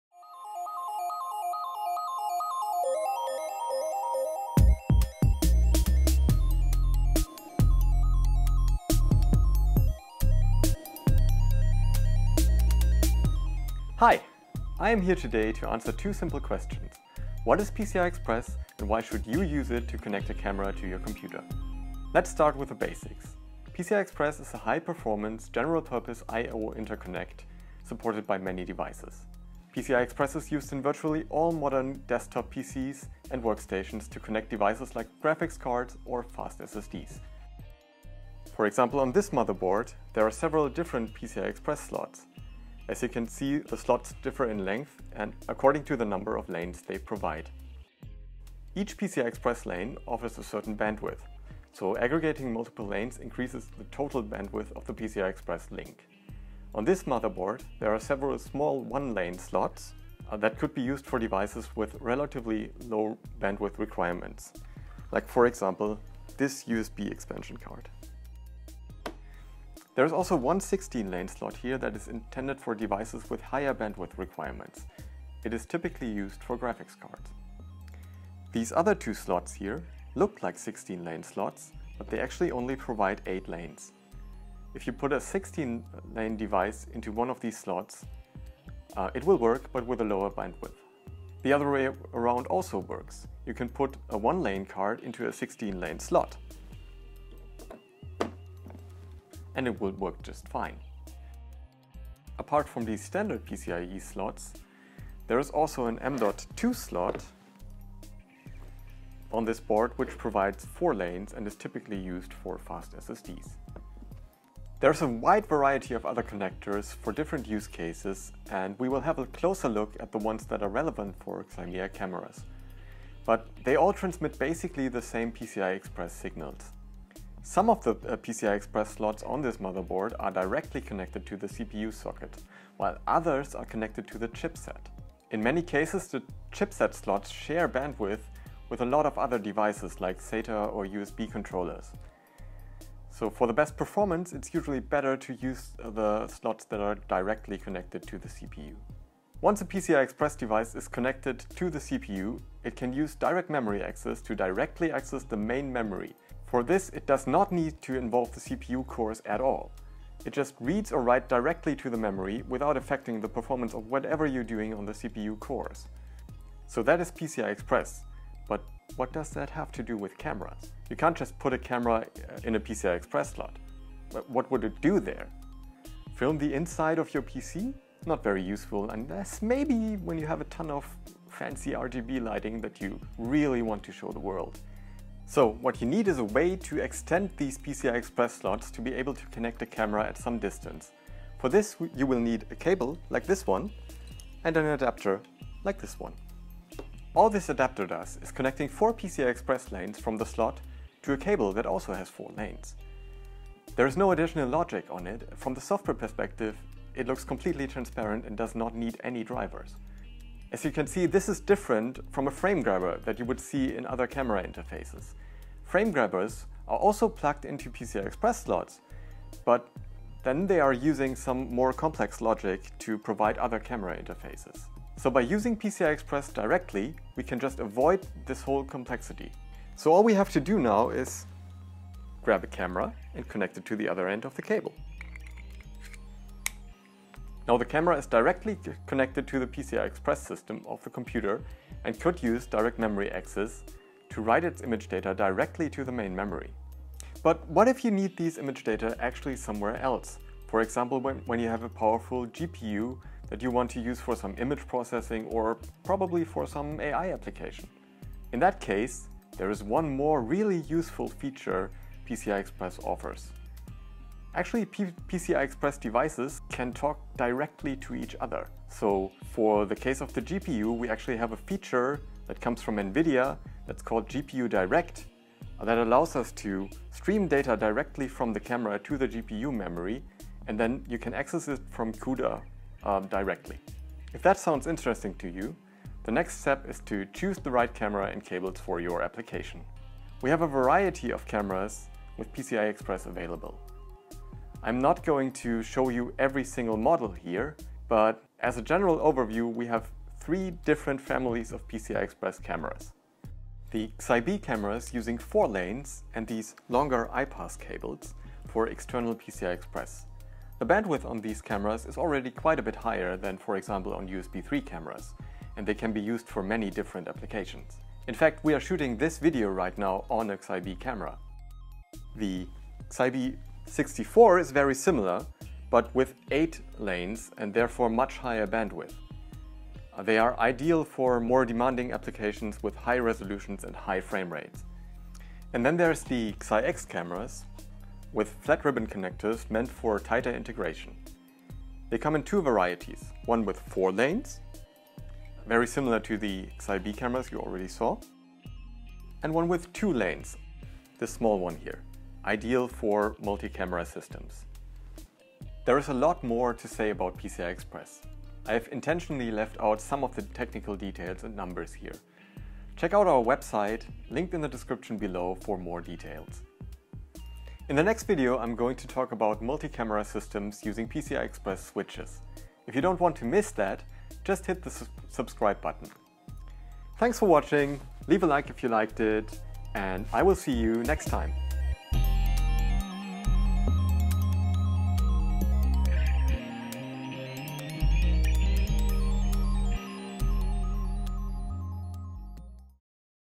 PCIe Interface - Advantages for XIMEA's industrial cameras and their high-bandwidth demands
Alex, Technology Expert at XIMEA, explains everything you need to know about PCIe and how you can use it to connect a high-performance camera directly to your system.